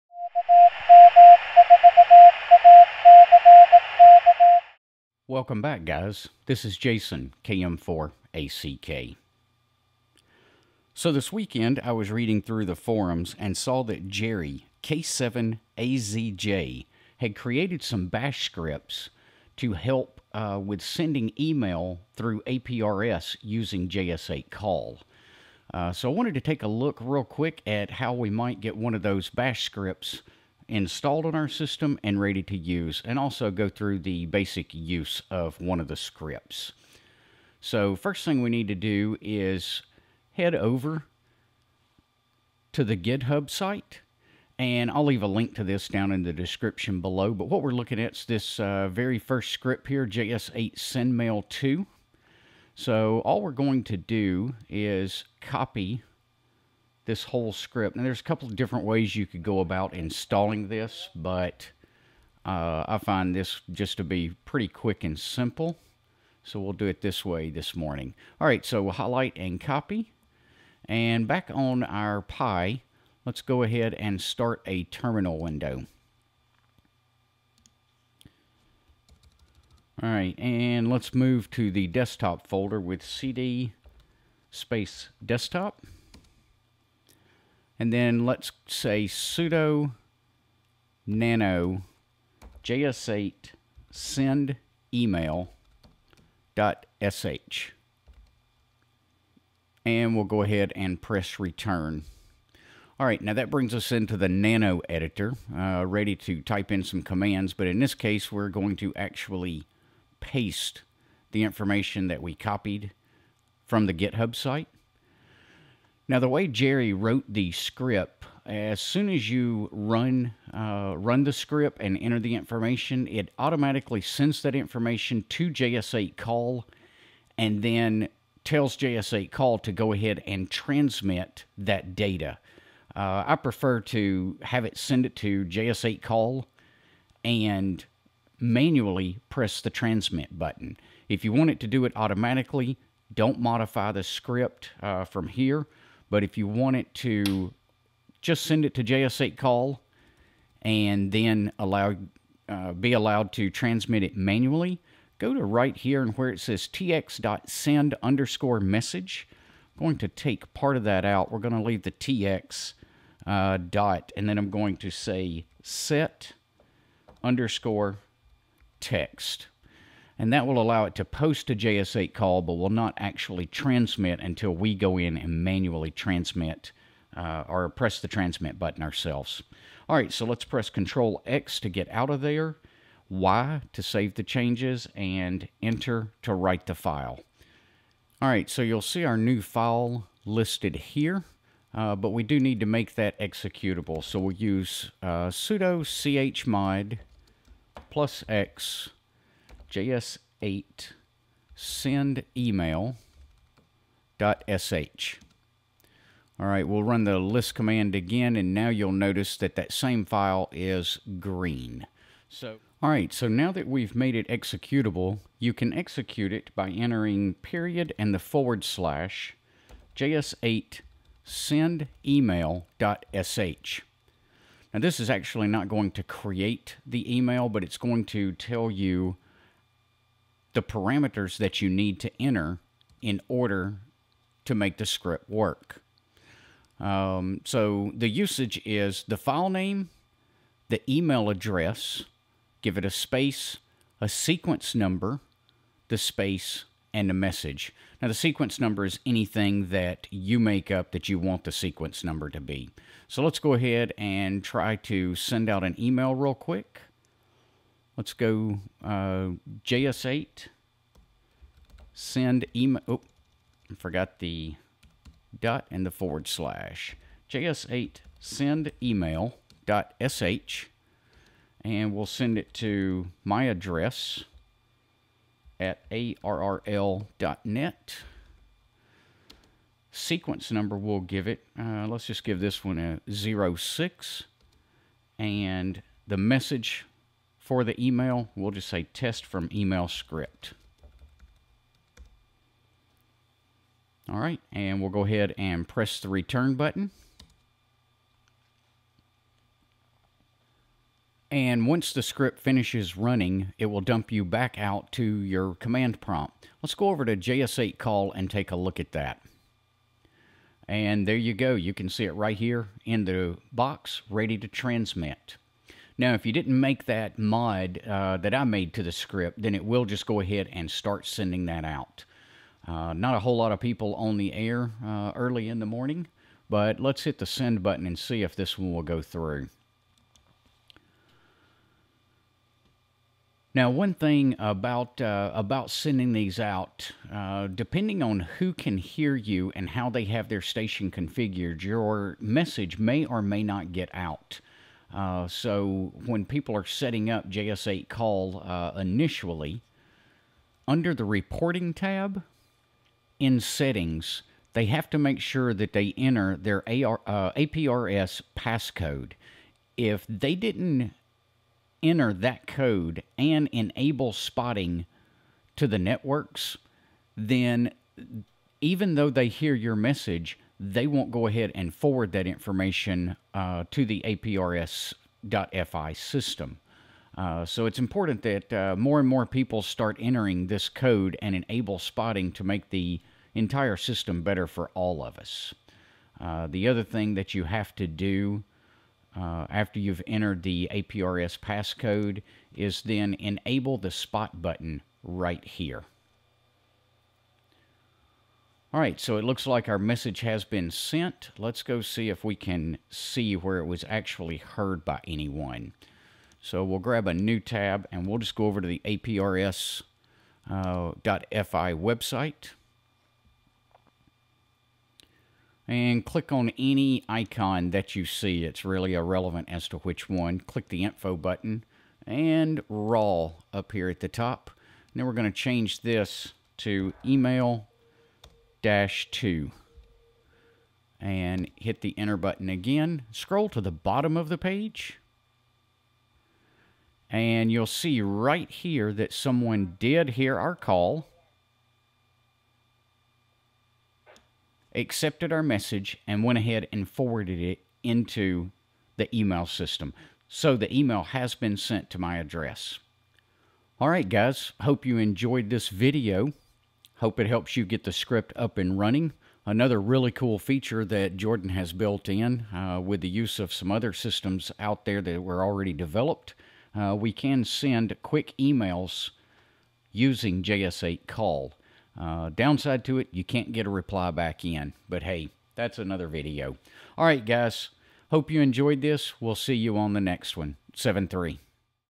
JS8Call Email Bash
Installing and using the email bash script written by Jerry, K7AZJ. Thanks Jerry for a great script. You can find Jerry’s scripts at

Jerry's Bash script code:

#!/usr/bin/env bash

if [ "$#" == "0" ]; then
  echo "usage: js8sendemail2 email@address seq-no message"
  echo "   example: js8sendemail2 email@address 03 hello"
  exit 0
fi

addr=$1
shift
seq=$1
shift
msg="$*"

echo $addr
echo $seq
echo $msg

value="\"@ALLCALL APRS::EMAIL-2  :${addr} ${msg} {${seq}}\""
echo sending $value

printf '{"params": {}, "type": "TX.SEND_MESSAGE", "value": %s}\n' "${value}" | nc -l -u -w 10 2237

73,
KM4ACK